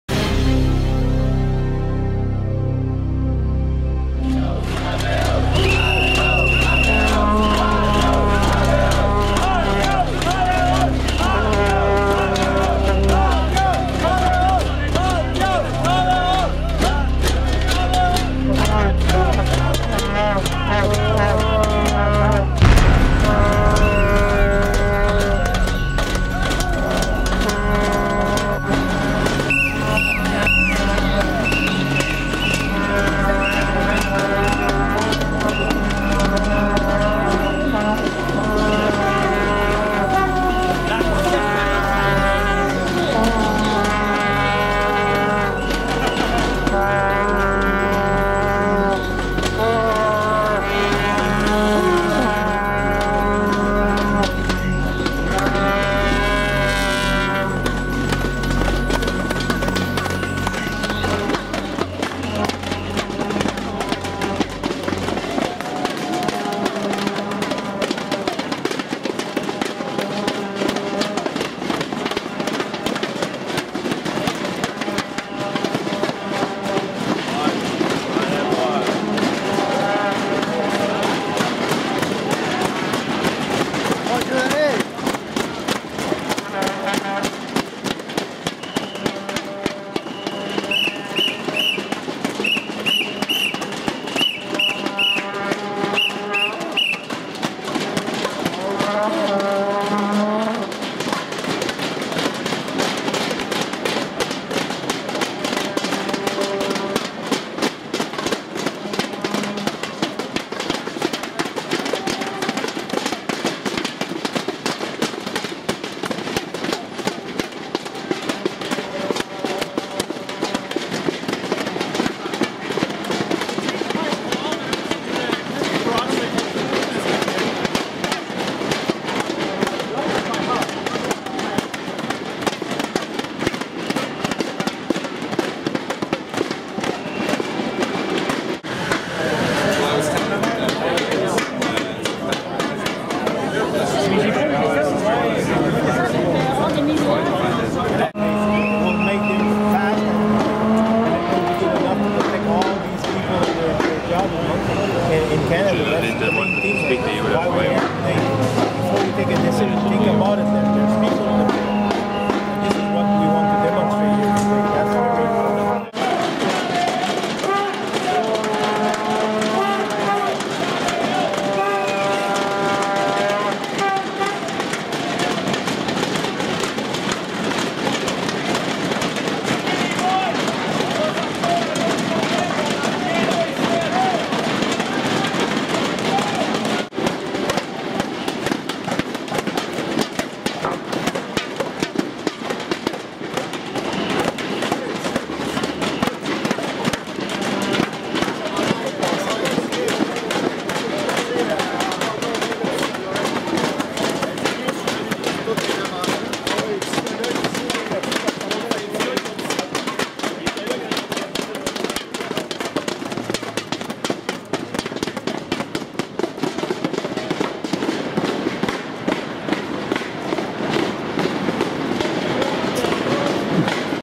IAMAW members demonstrate outside CIRB offices in Ottawa
There was a strong showing on Monday October 4, 2010 outside the offices of the Canadian Industrial Relations Board in Ottawa (CIRB) by hundreds of members of the International Association of Machinists and Aerospace Workers (IAMAW) -- Air Canada employees who were seconded to Aveos Fleet Performance Inc.